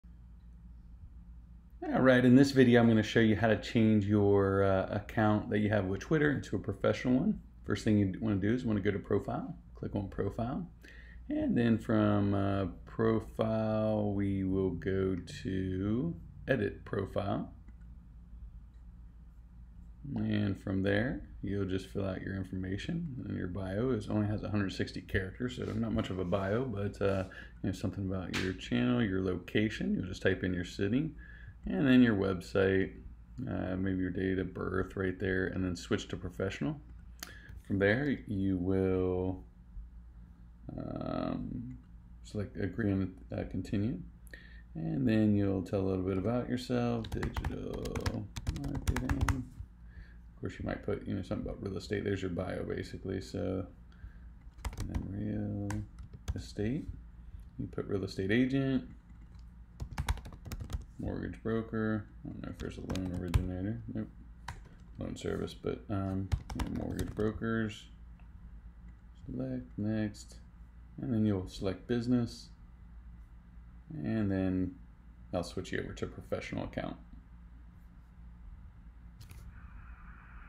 How to change Twitter to a professional account (2023)
This video I'll go over how to switch your Twitter account to a professional account.